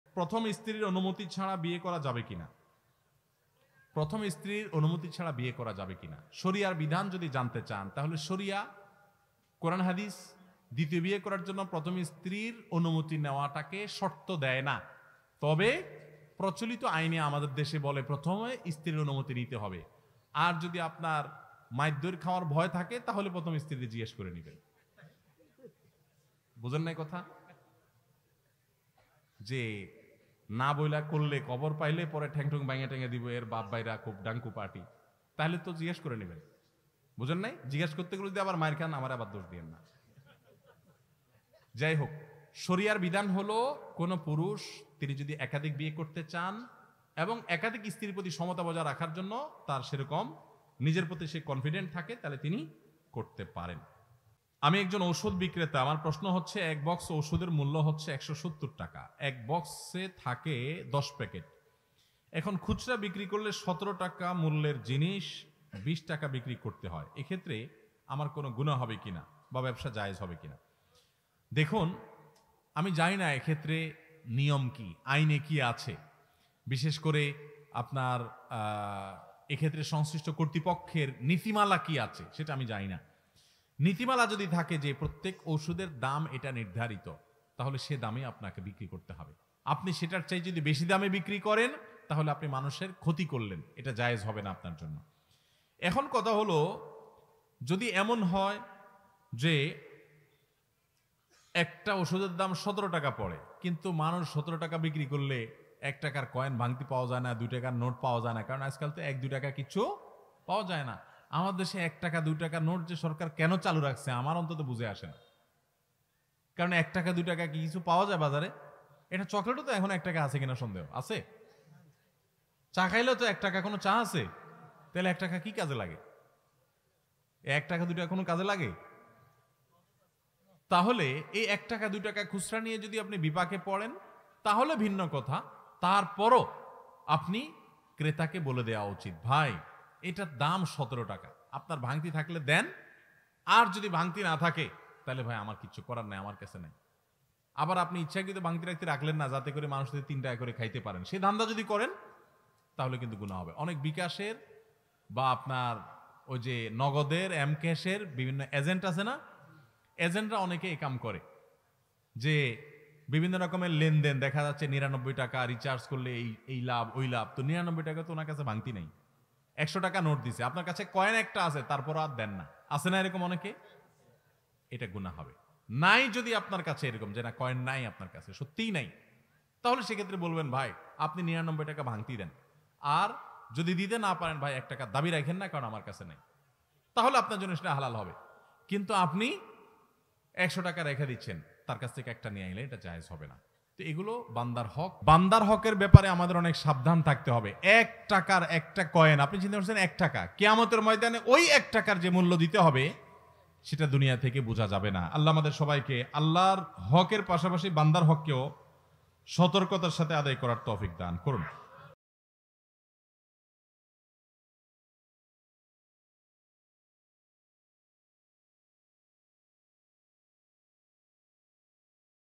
দ্বিতীয় বিয়ে করতে প্রথম স্ত্রীর অনুমতি নেয়া কি জরুরী?
দ্বিতীয় বিয়ে করতে প্রথম স্ত্রীর অনুমতি নেয়া কি জরুরী? new waz II waj II Sheikh ahmadullah